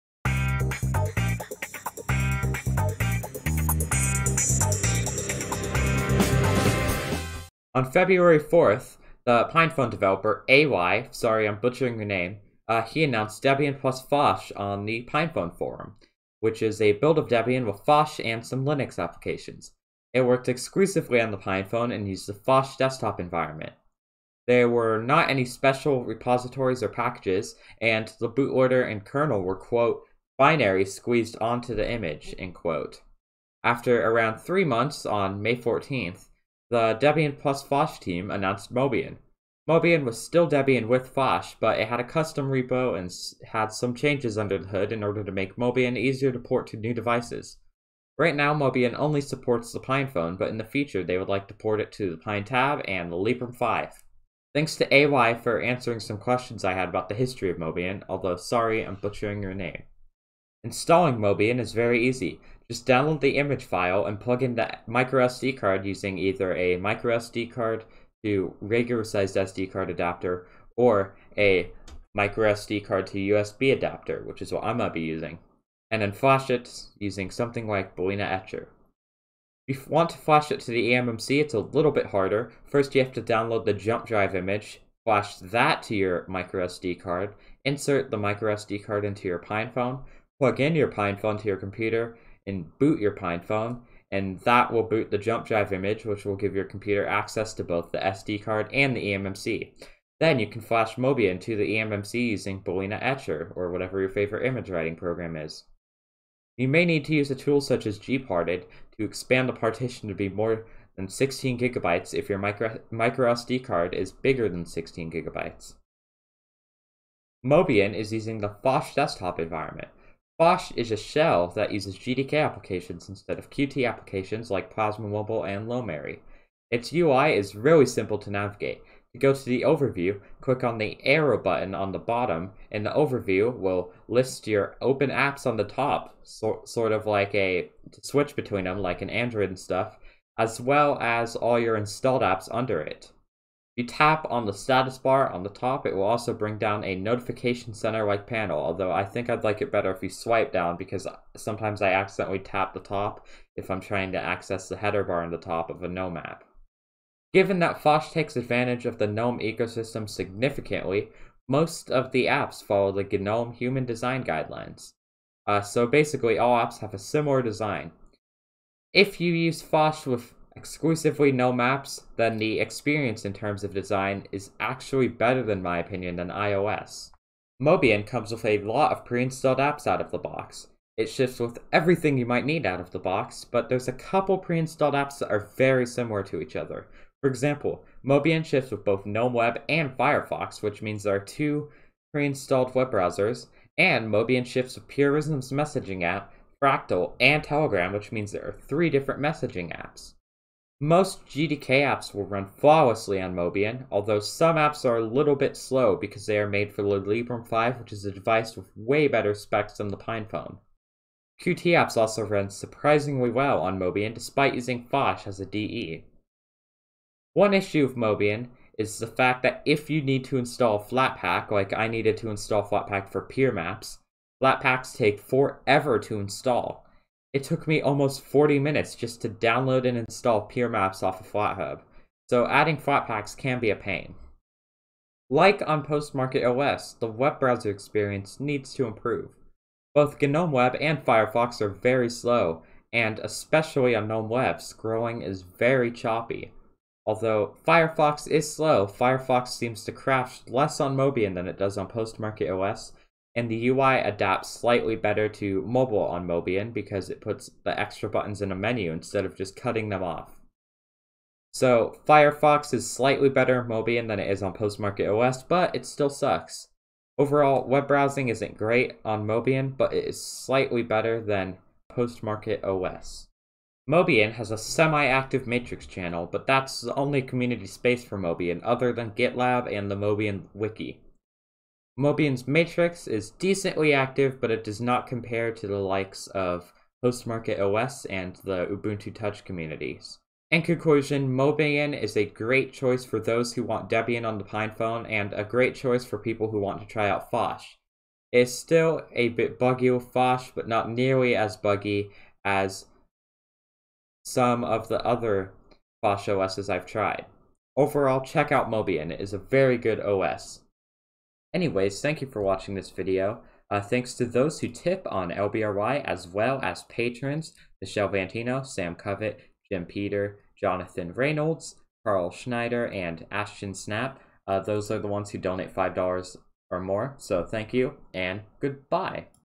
PinePhone OSes: Mobian
I believe Mobian is one of the best distros for the PinePhone.

Timestamps:
0:00 Intro and History
1:00 Installation
1:57 UI
2:37 Apps
3:55 Web Browsing
4:33 Community
4:50 Outro I guess

If you see this, comment:
"Grub is better then SystemD"